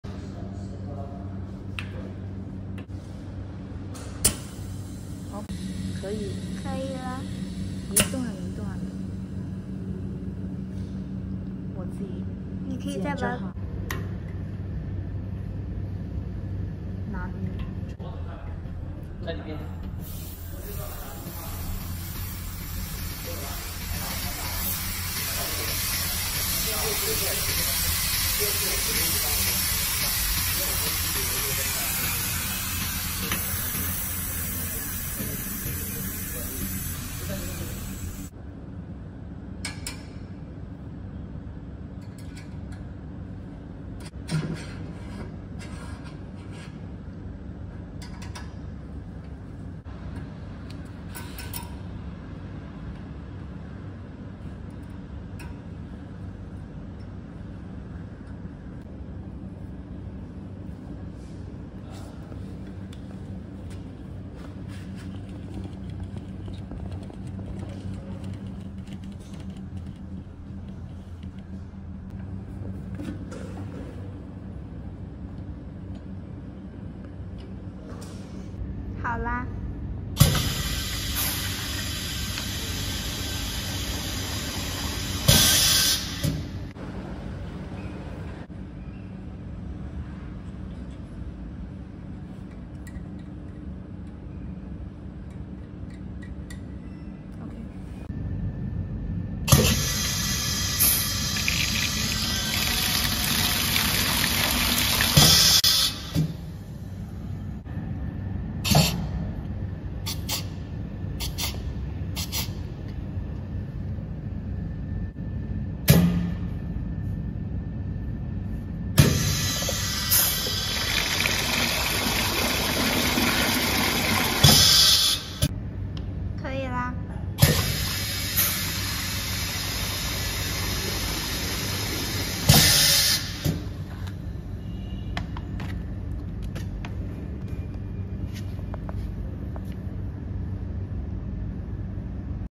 How to use horizontal semi automatic pneumatic piston filling machine manual operation video
Full pneumatic driven type
Filling volume option: 10-100ml, 20-250ml, 50-500ml, 100-1000ml.